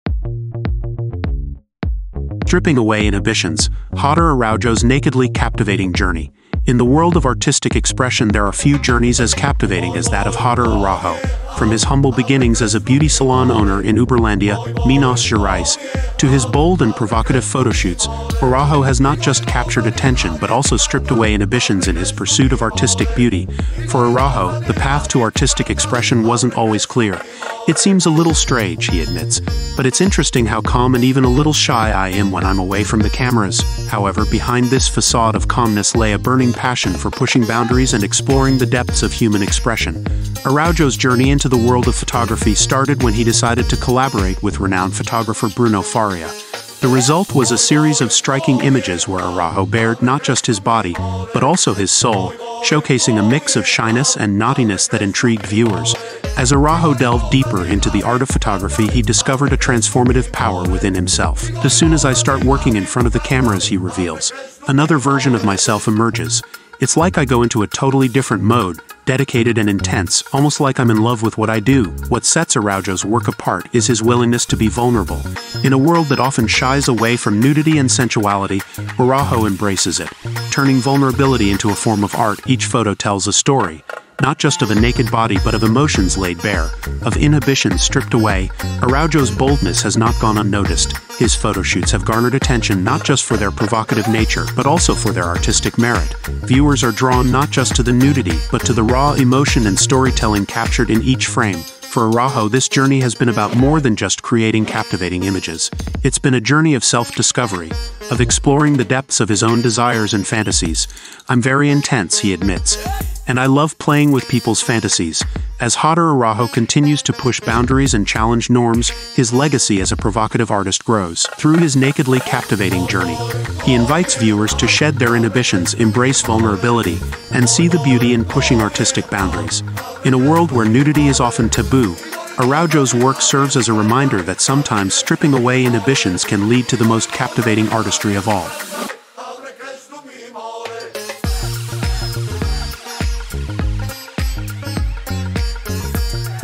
Stripping Away Inhibitions: Jader Araujo's Nakedly Captivating Journey!
Jader Araujo's journey from a beauty salon owner to a provocative model and photographer is a tale of stripping away inhibitions and embracing vulnerability. Collaborating with photographer Bruno Faria, Araujo discovered a transformative power within himself, showcasing a mix of shyness and naughtiness in his bold photoshoots. His work challenges norms and invites viewers to shed inhibitions, embracing the beauty of pushing artistic boundaries. Araujo's legacy lies in his ability to capture raw emotions and tell captivating stories through his art, proving that vulnerability can be a powerful form of self-expression.